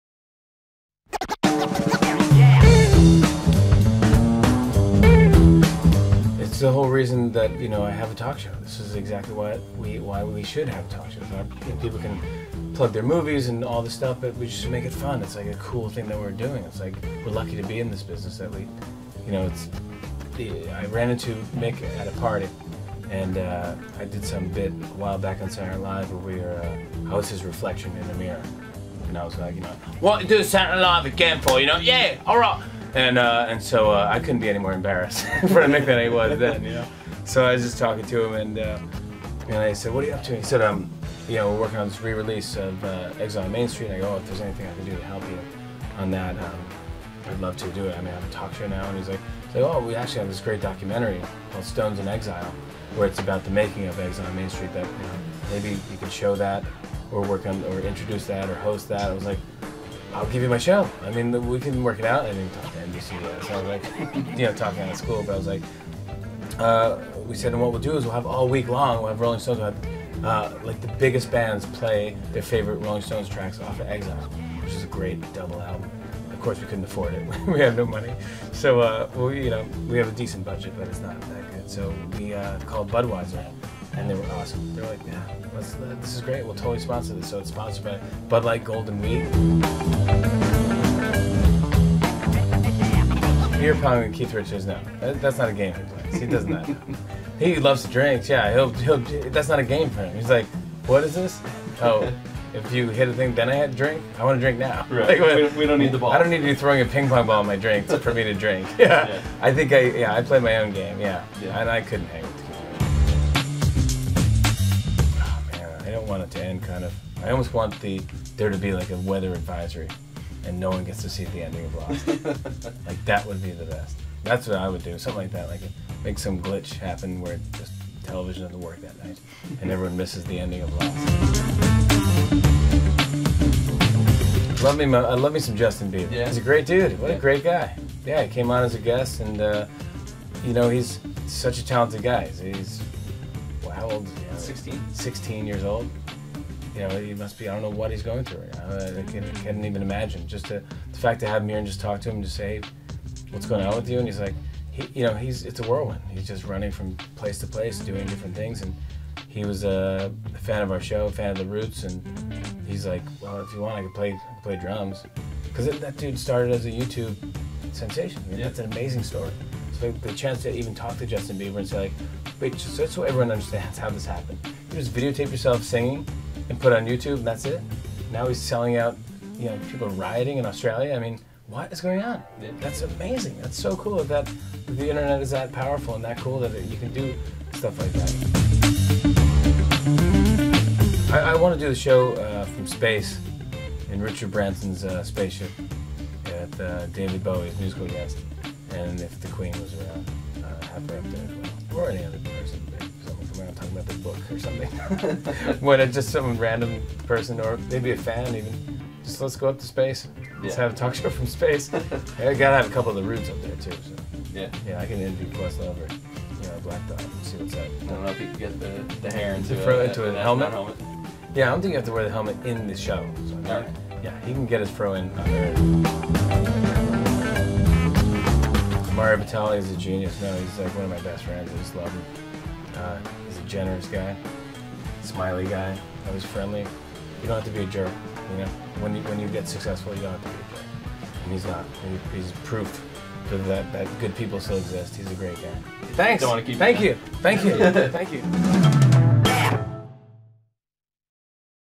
Jimmy Fallon Talks Stones, 'Lost' Finale and Bieber
PopEater was able to catch up with our pal Jimmy to discuss the Stones, Keith Richards' drinking habits ("I couldn't hang with Keith," Jimmy admits to us), the impending 'Lost' finale and more. Watch the exclusive interview!